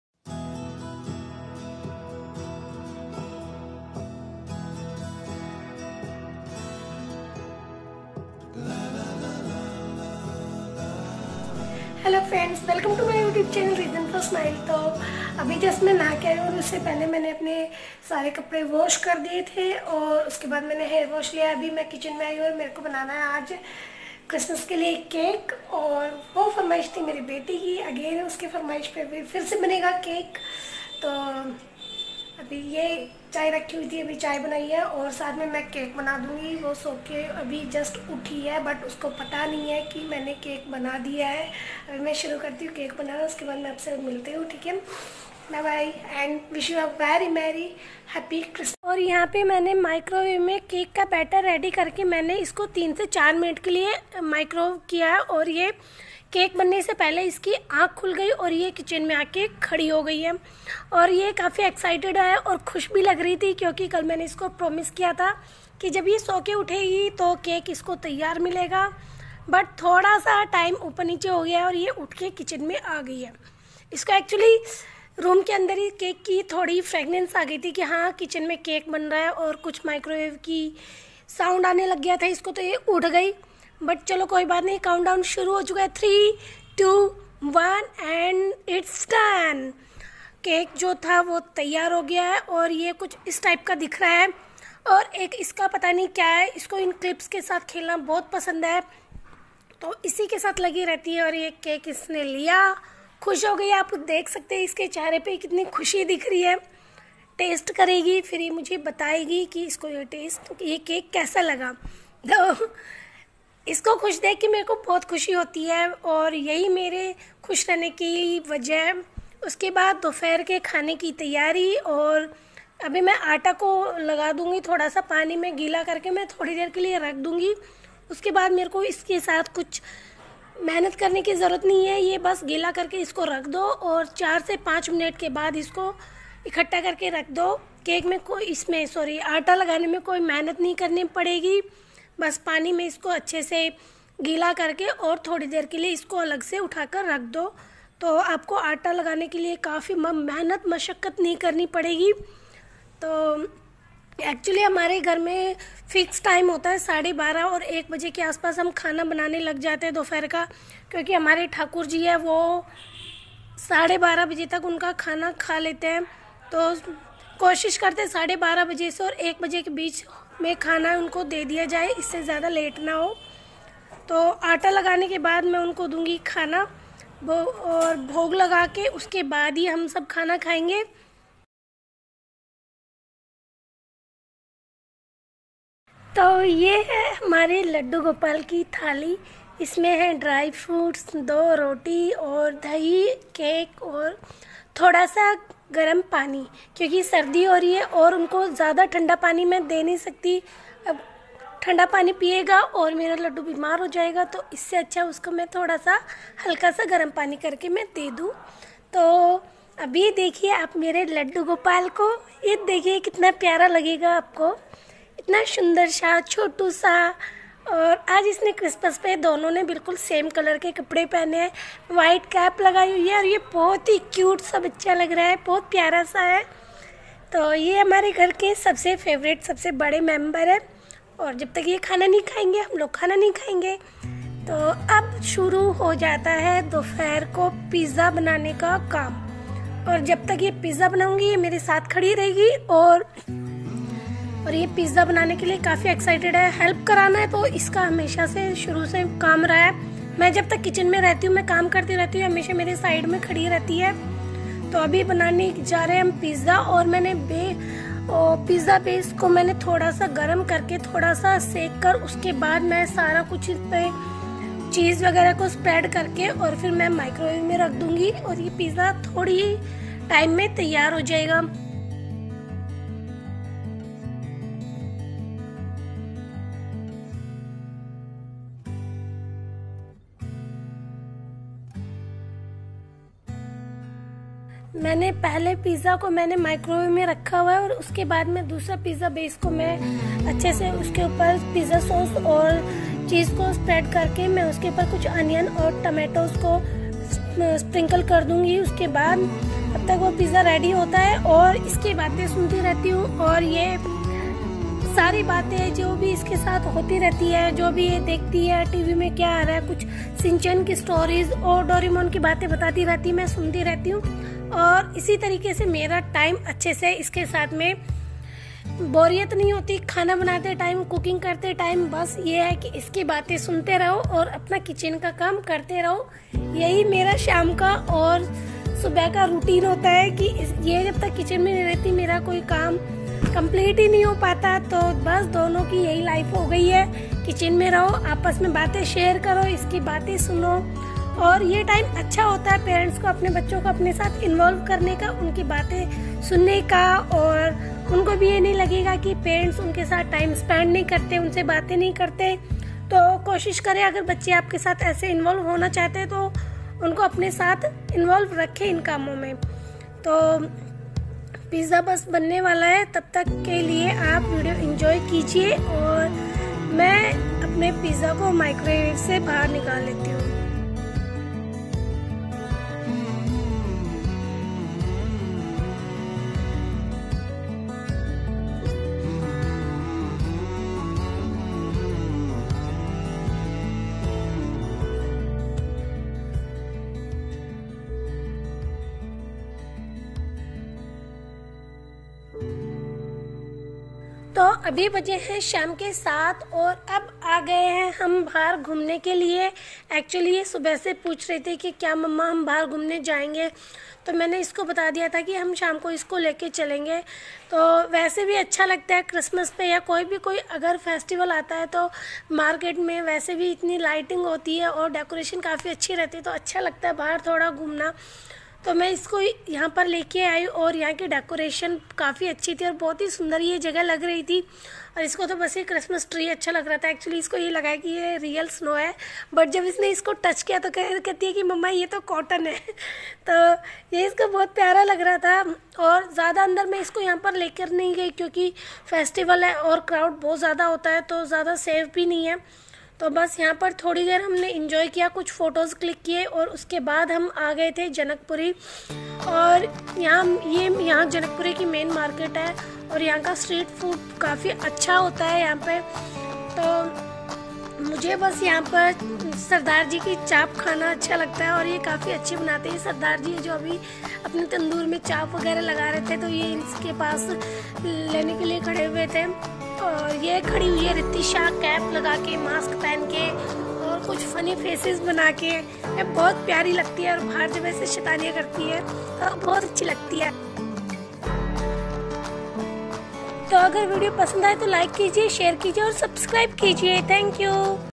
christmas vlog ,kaise kiya aaj celebrate Christmas or kya khaas hua
christmas vlog,yummy homemade Pizza cake on this day, and lots of fun with Ritisha 🎂🍰❤️